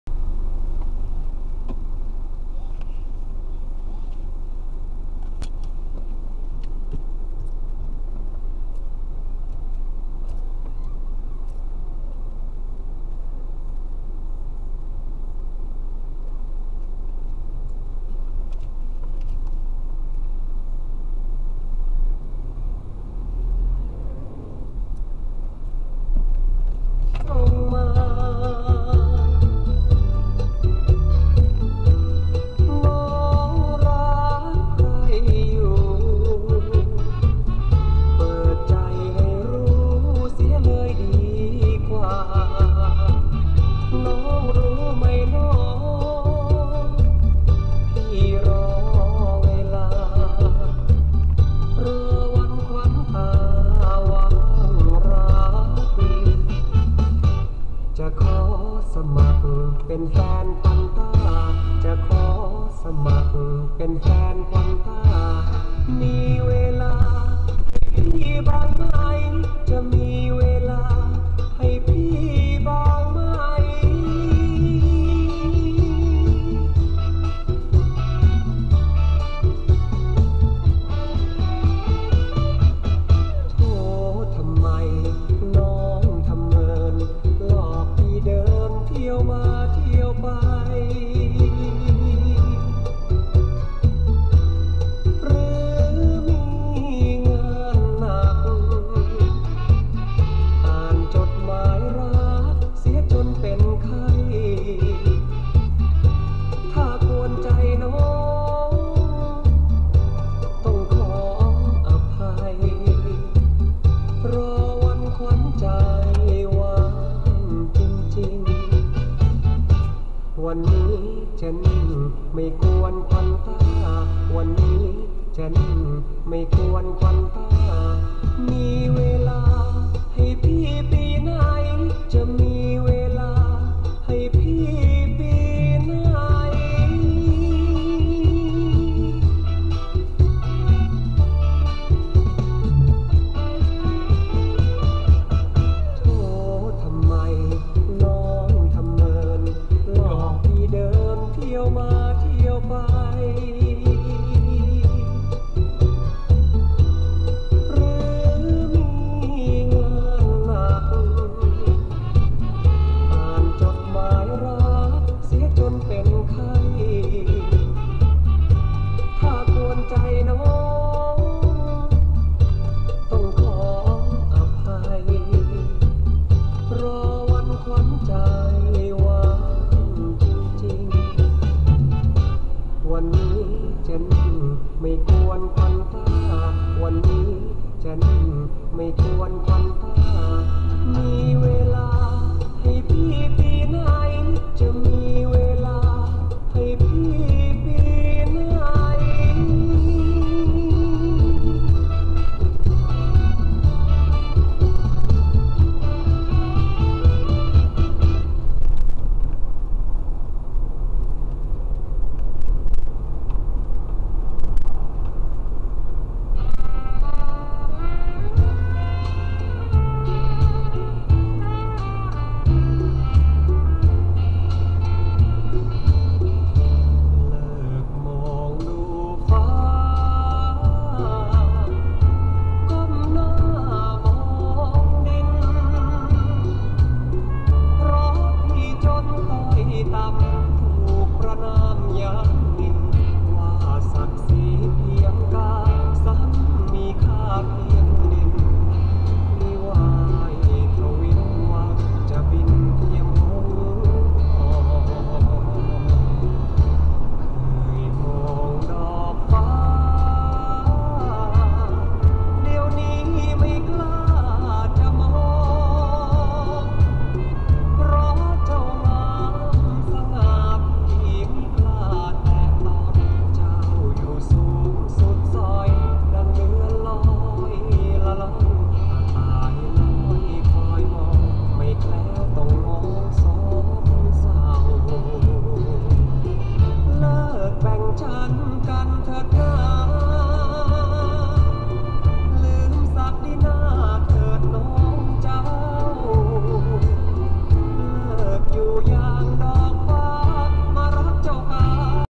ฟังเพลงเบาๆยามเดินทาง กลับเลิกงาน
ฟังเพลงเบาๆยามเดินทาง มาทำงานปกติ ชีวิตก็มีแค่นี้แหละคนเรา ทำงาน กลับบ้าน กินข้าว งานบ้าน งานเรือน งานเพื่อนพี่น้อง เพื่อนบ้าน เลี้ยงสัตย์ไว้ดูเล่น ความสุขอยู่ที่เรา เราสร้างมัน ด้วยมือเราและสิ่งช่วยจากสิ่งสร้างเรามาบนโลกนี้